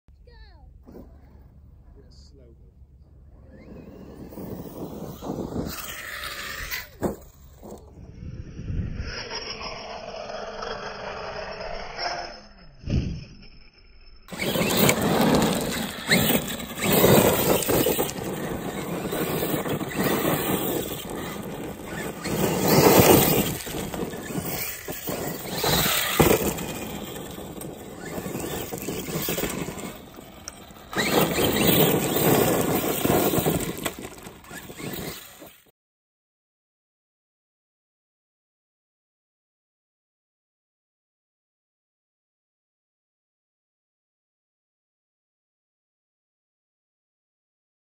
Bash It - Crash It - Sent It - Bend It - Mend It !! Arrma Kraton 6S V4
Nothing broke, just the A-arm ball joint popped out of the hub (1st time thats happened) but fitted it back together fine :)